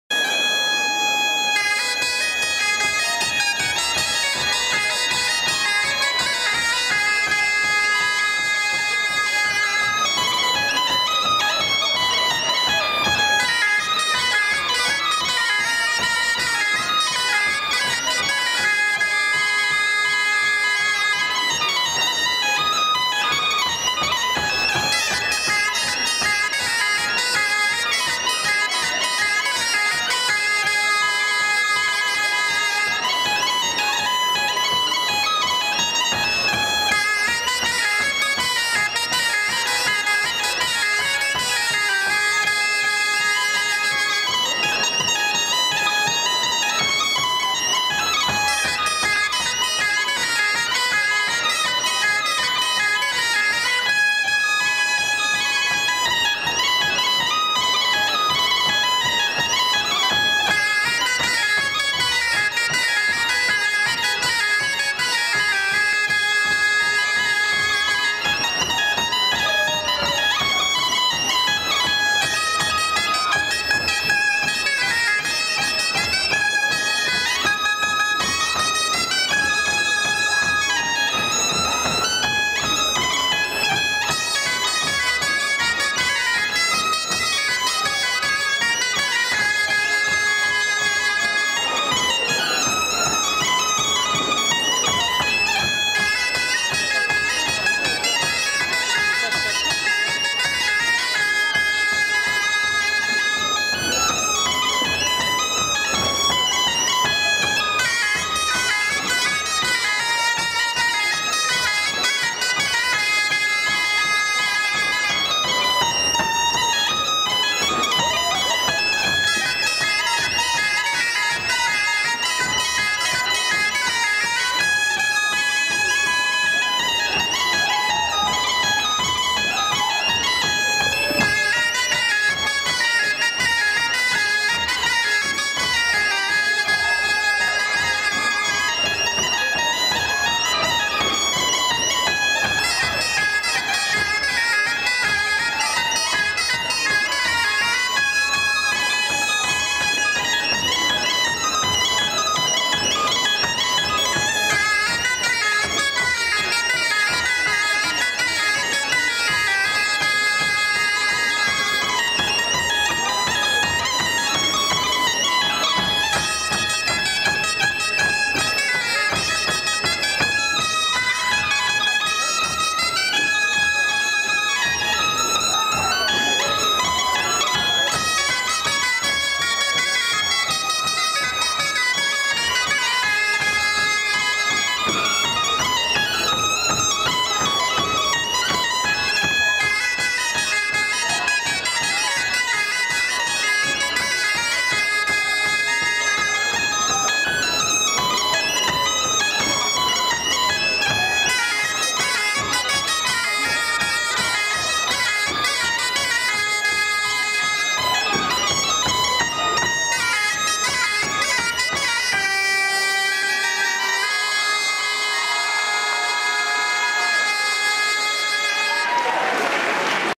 Duo Biniou-Bombarda Lorient 2008
Interceltic Festival Lorient August 2008 - Trad Breton music - FEST NOZ